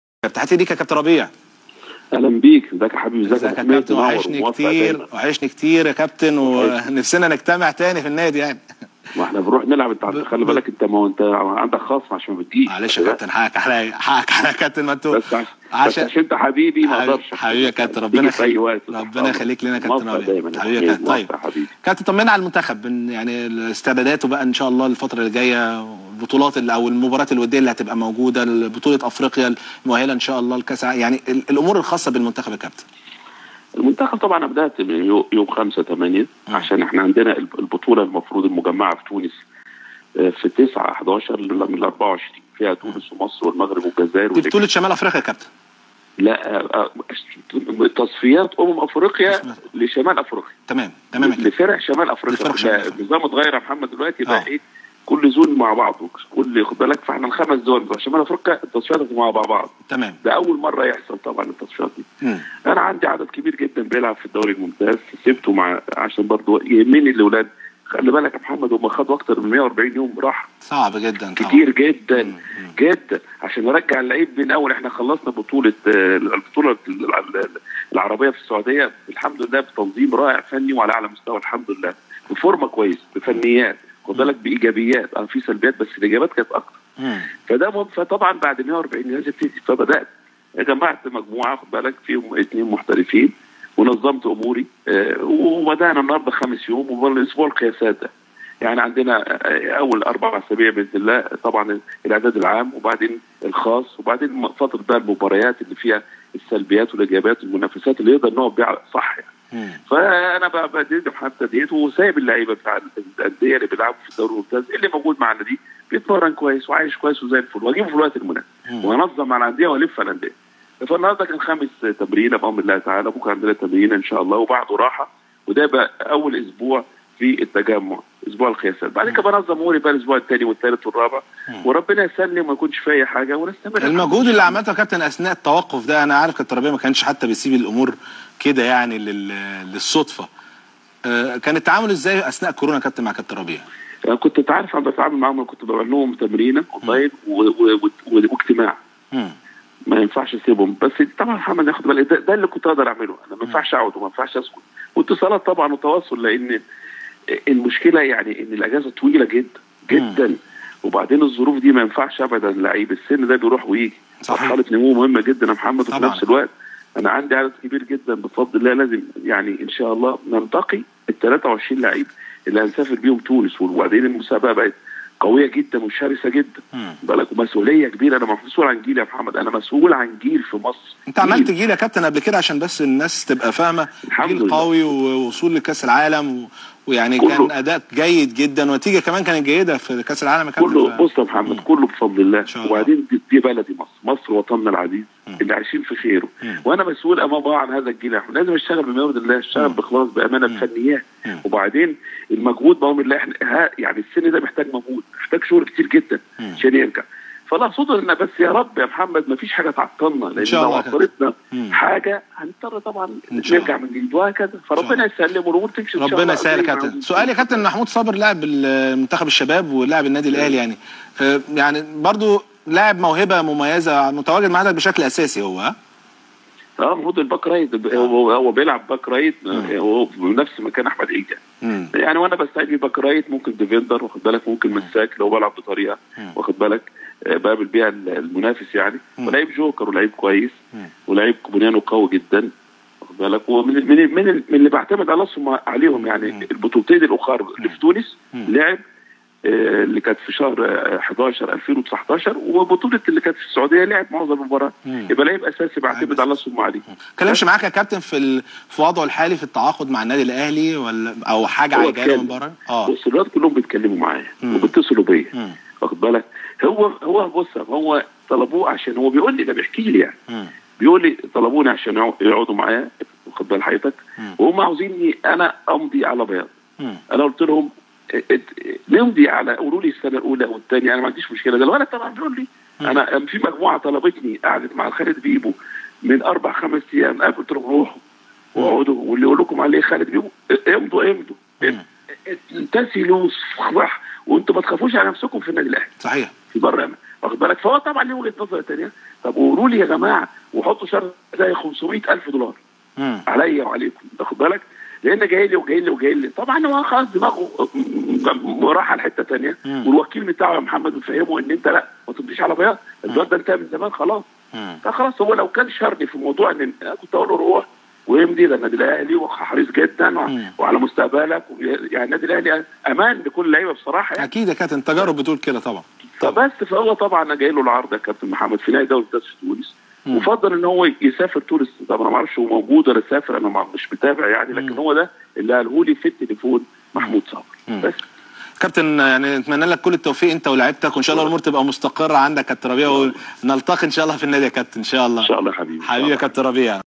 ربيع ياسين عن محمود صابر : الاهلي كان عايز يخليه يمضي على بياض و هو لاعب جوكر واعتمد دايما عليه
الاهلي كان عايز يخليه يمضي على بياض و هو لاعب جوكر واعتمد دايما عليه
ربيع ياسين عن محمود صابر